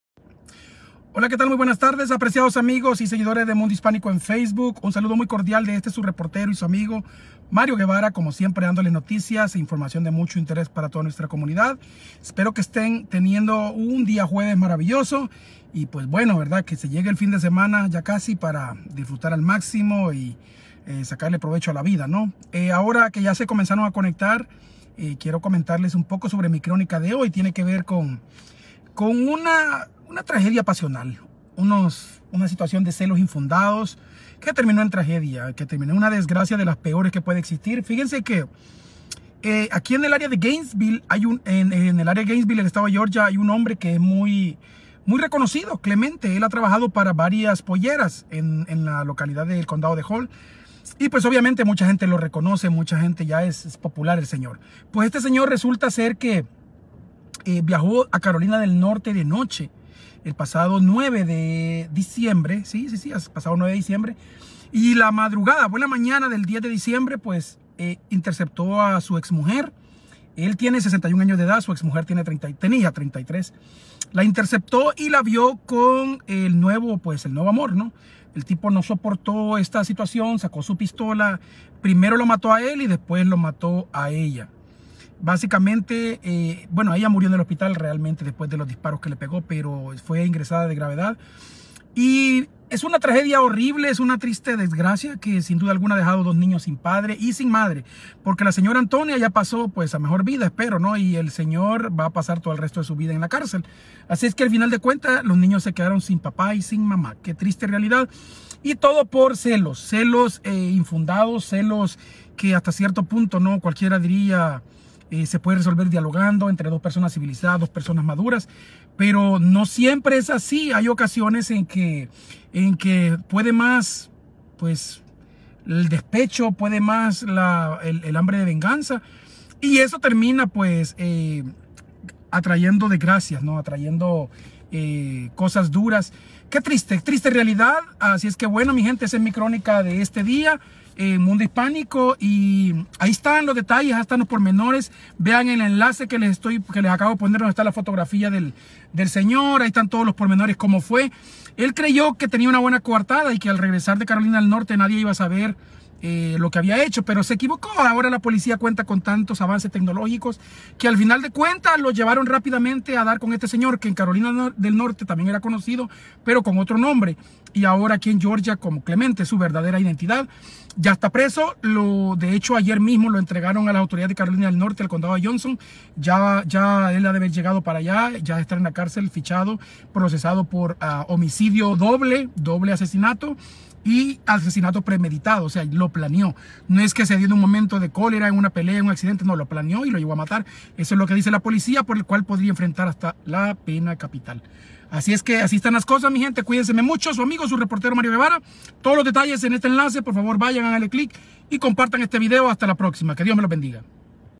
Hispano de Georgia viajó a otro estado para matar a su exmujer por haberlo abandonado Noticias
Crónica: Hispano viaja solo para matar a su exmujer. Creyendo que tendría una buena coartada para salirse con la suya, un hispano de Georgia viajó de noche y sin avisarle a nadie hasta el estado vecino de Carolina del Norte para cobrarle caro a su exmujer y madre de sus dos hijos el hecho de haberlo abandonado por irse con otro individuo.

Pero Clemente Hernández Mojica no contaba con que las autoridades estadounidenses tienen métodos extremadamente avanzados para investigar casos y determinar responsabilidades y fue exactamente, así como lograron cacharlo, a pesar de que allá medio mundo lo conocía, pero con otra identidad.